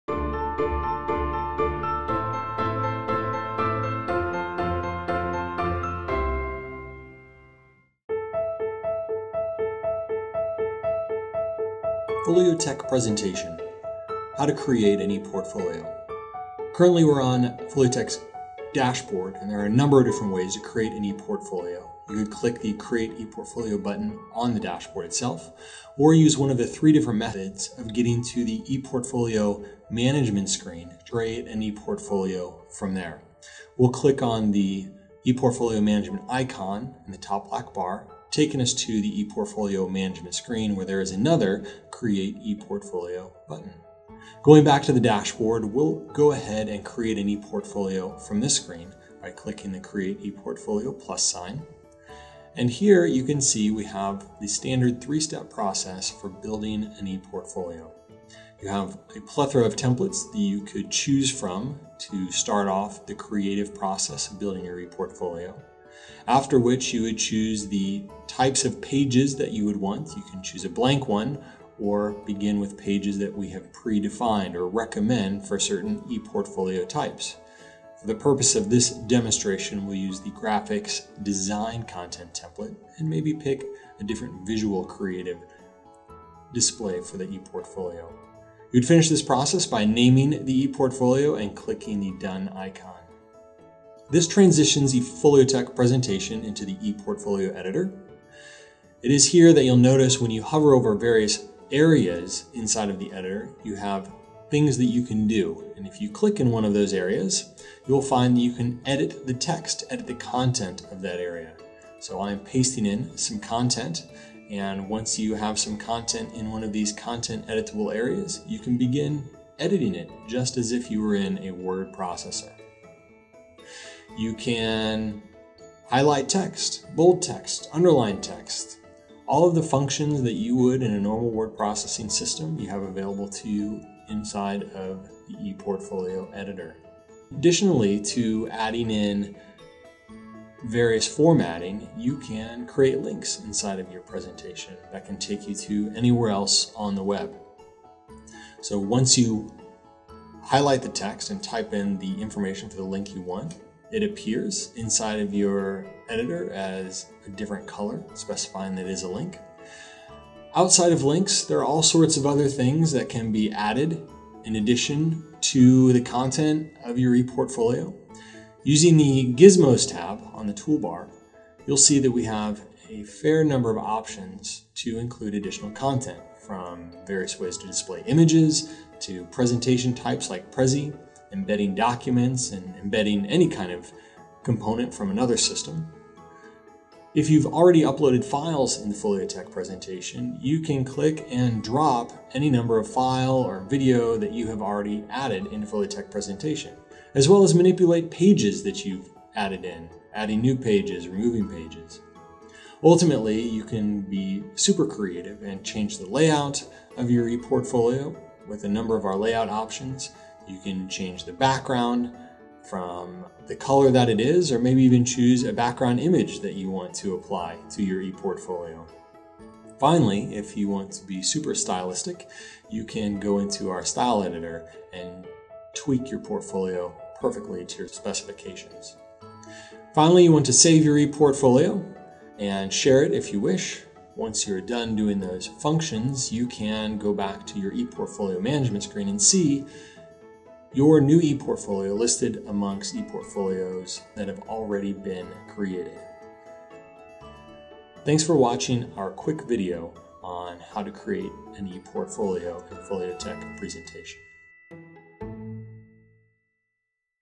Foliotek Presentation - Create ePortfolio
Learn how to create an ePortfolio in Foliotek Presentation